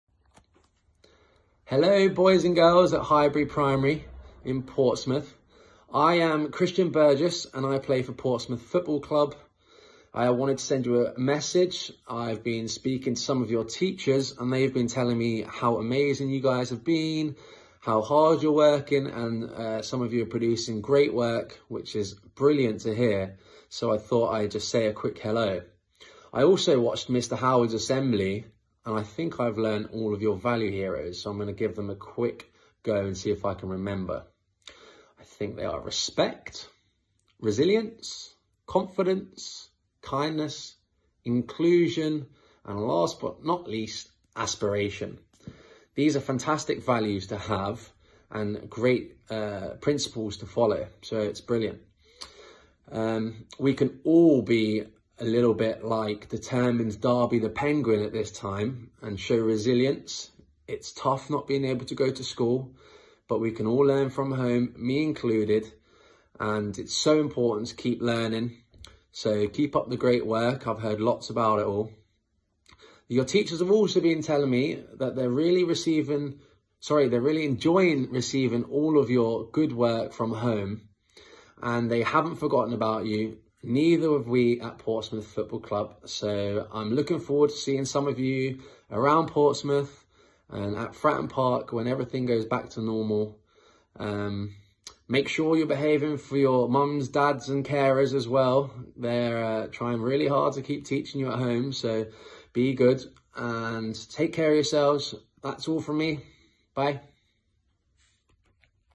Christian Burgess, Portsmouth Football Club superstar, shares a special message.
Christian Burgess asks us all to be a ‘Determined Darby’ and show resilience.